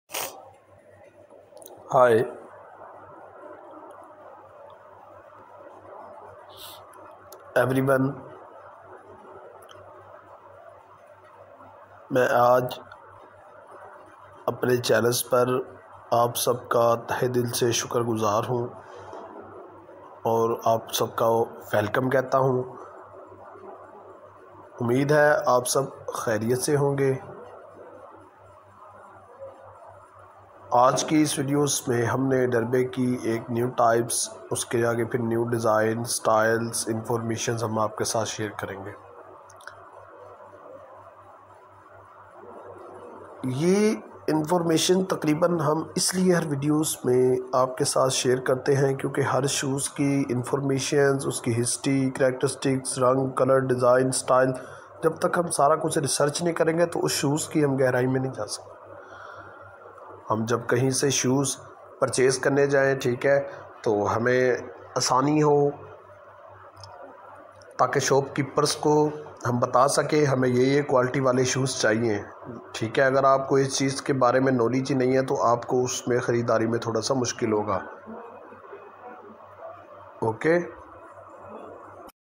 Color full Stylish derby shoes.
color full Stylish derby shoes different types and different design and complete information is inside this video.